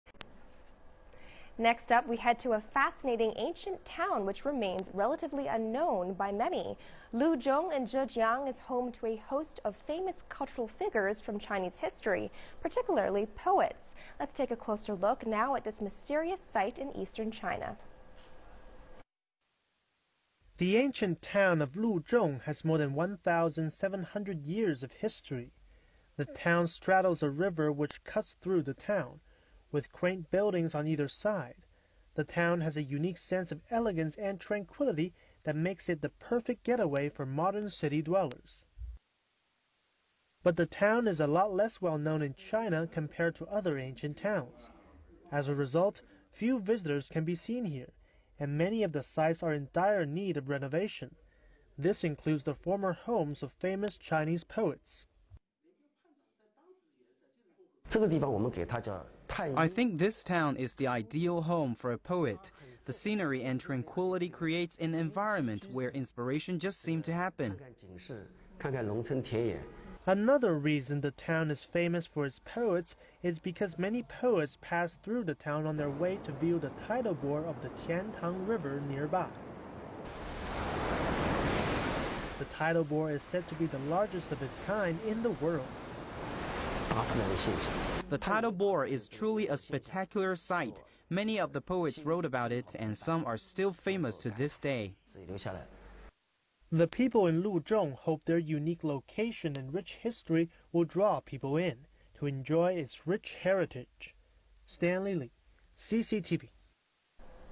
Cultural and literary treasures of an ancient town in Zhejiang
A fascinating ancient town in east China's Zhejiang province remains relatively unknown by many. Luzhong is home to a host of famous cultural figures from Chinese history, particularly poets.
The ancient town of Luzhong has more than 1,700 years of history. The town straddles a river, which cuts through it with quaint buildings on either side. The town has a unique sense of elegance and tranquillity that makes it the perfect getaway for modern city dwellers.